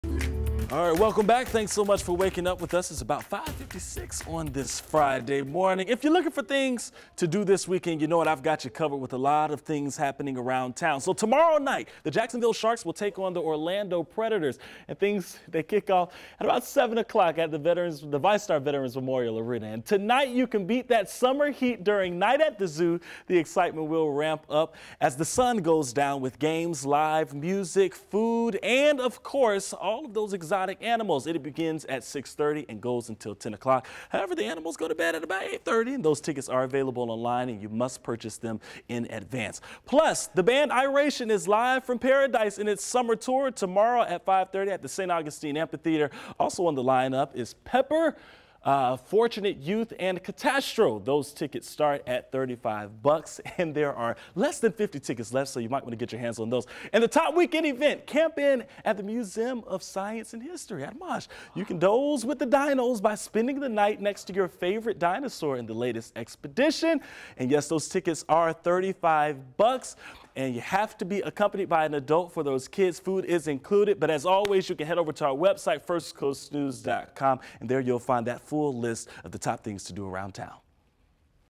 First Coast Weekend Events!
Sharks Game, Night at the Zoo, Iration and much more!